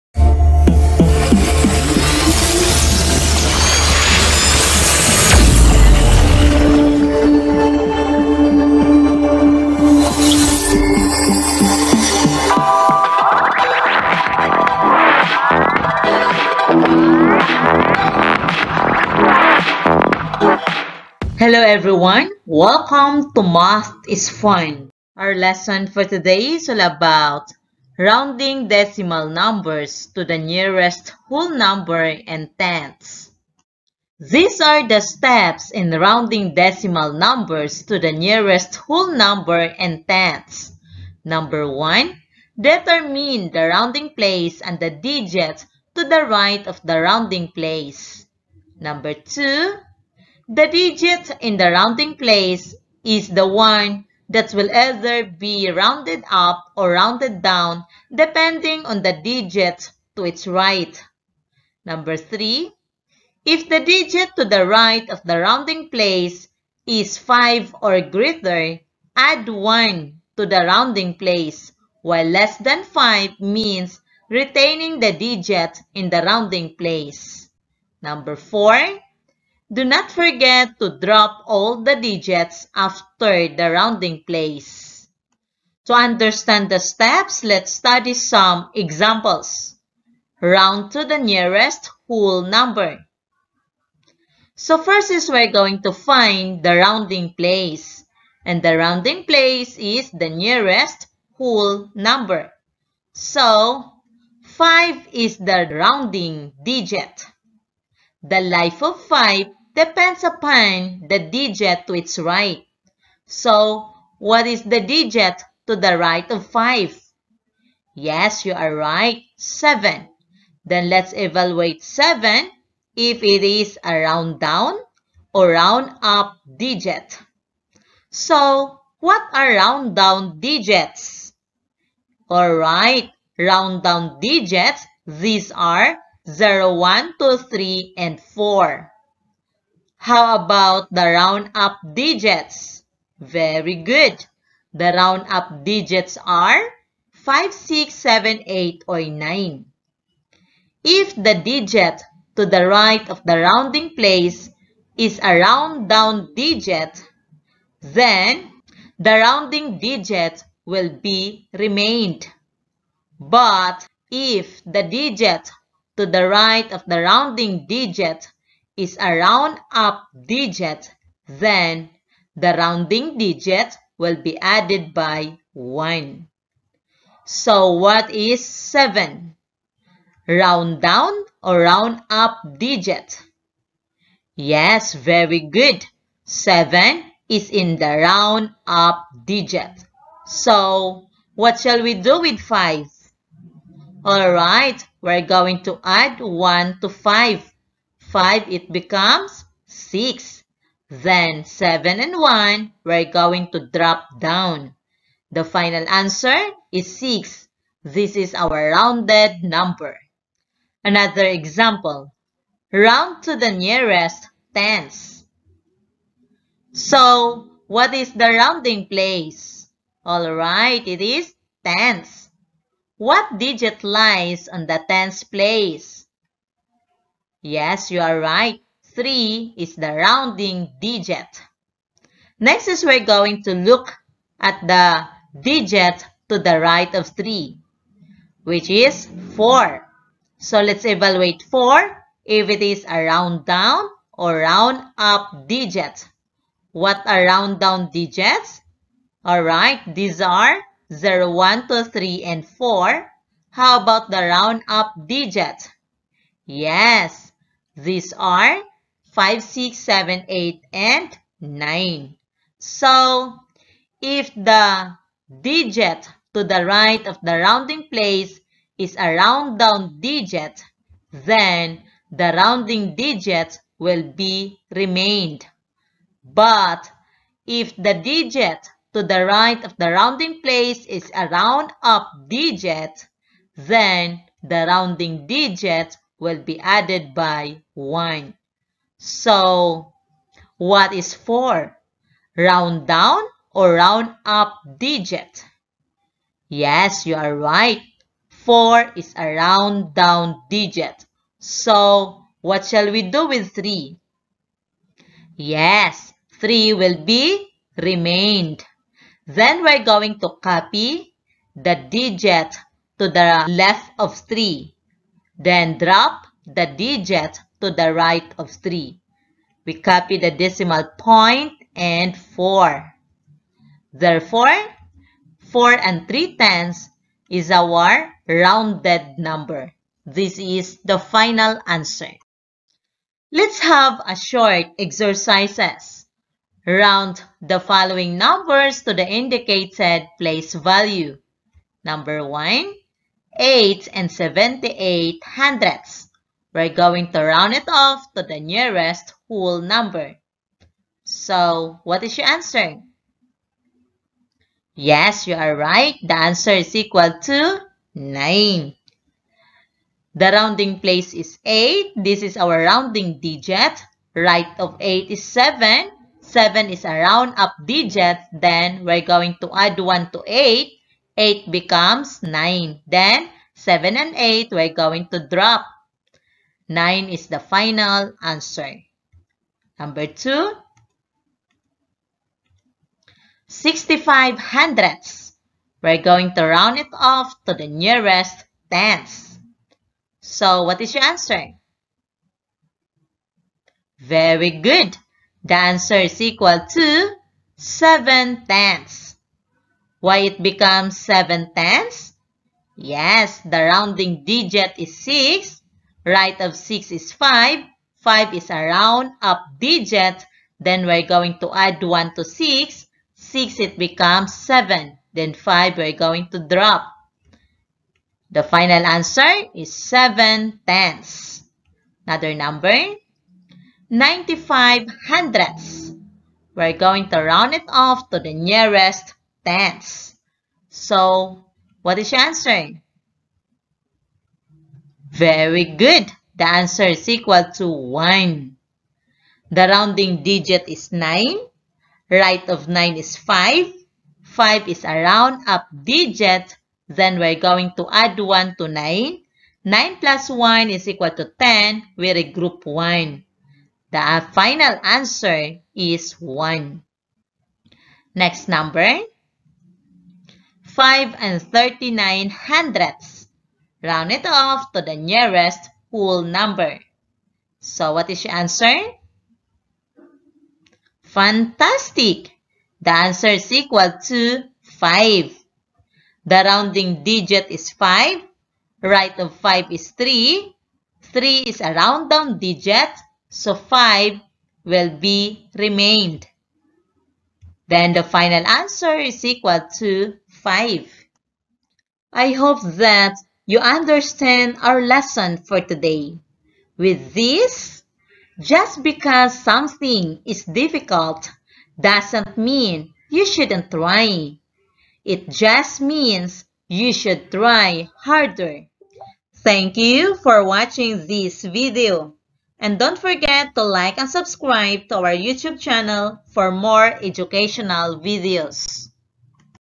Math 4 Q2 Rounding Decimal Numbers to the Nearest Whole Number and Tenths
This video is all about rounding decimal numbers to the nearest whole number and tenths.